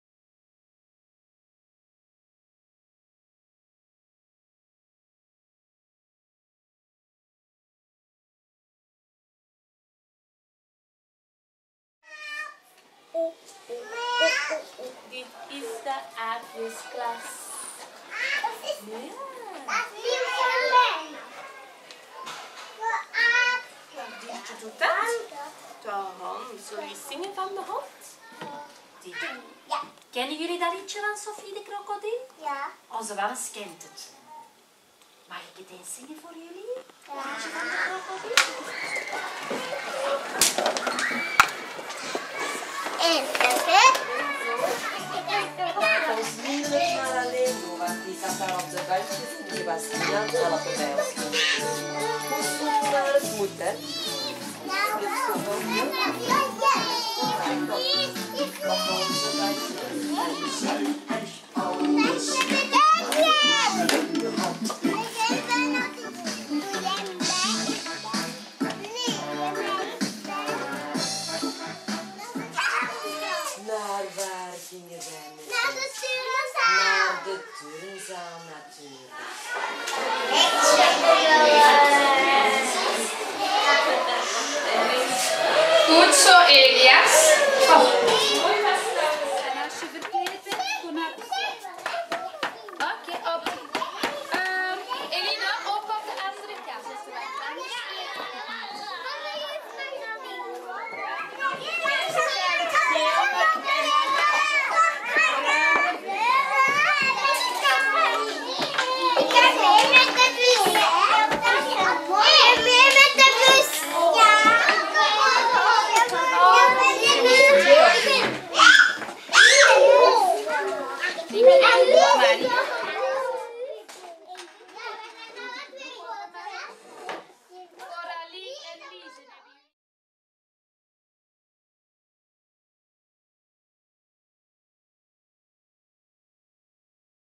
Binnenkijken in de klasjes van de 2,5- en 3-jarigen (oktober 2011)
Binnenkijken in de klasjes van deze kapoenen te Olleke Bolleke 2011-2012